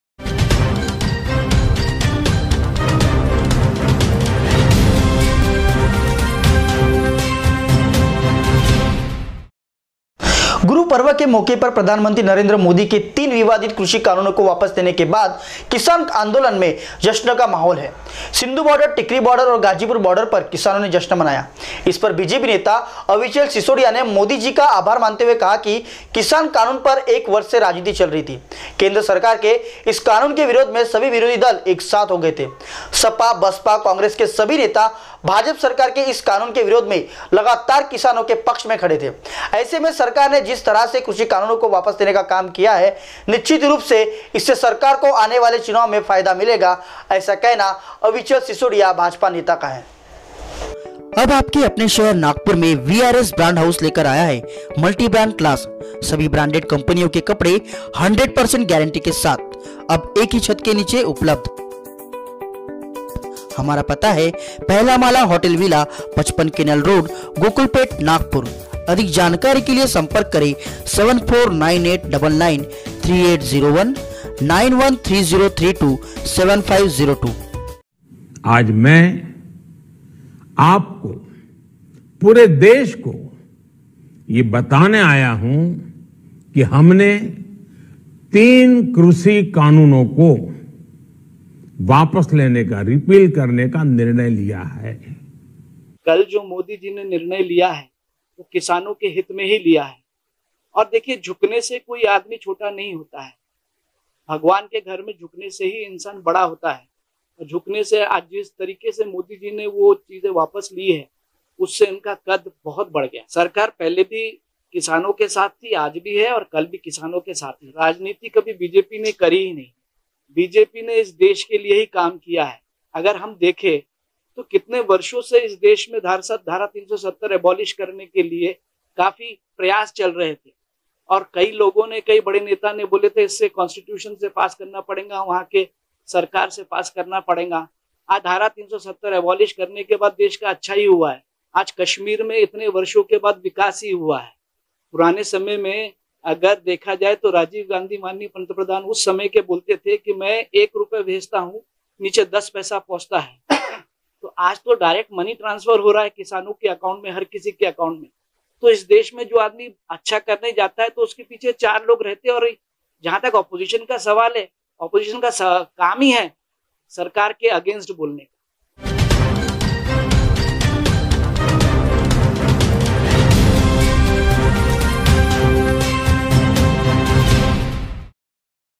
मोदी जी के फैसले से उनका कद और बढ़ गया है- अविचल सिसौडीया
गुरू पर्व के मौके पर प्रधानमंत्री नरेंद्र मोदी के तीन विवादित कृषि कानूनों को वापस लेने के बाद किसान आंदोलन में जश्न का माहौल है। सिंघु बॉर्डर, टीकरी बॉर्डर और गाजीपुर बॉर्डर पर किसानों ने जश्न मनाया है। इस पर बीजेपी नेता अविचल सिसोडीया ने मोदी जी का आभार मानते हुए कहा कि किसान कानून पर 1 वर्ष से राजनीति चल रही थी । केन्द्र सरकार के इस कानून के विरोध में सभी विरोधी दल एक साथ हो गए थे। सपा, बसपा, कांग्रेस के सभी नेता भाजपा सरकार के इस कानून के विरोध में लगातार किसानों के पक्ष में खड़े थे। ऐसे में सरकार ने जिस तरह से कृषि कानून को वापस लेने का काम किया निश्चित रूप से इससे सरकार को आने वाले चुनाव में फायदा मिलेंग#farmersprotest